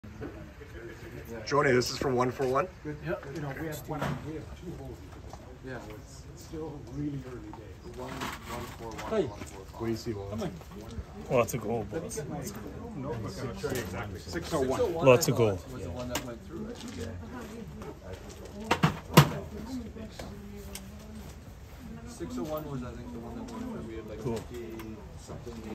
Previously announced drill hole TUG 141 High Grade Gold Discovery of a major new feeder structure
Connect with Lion One Metals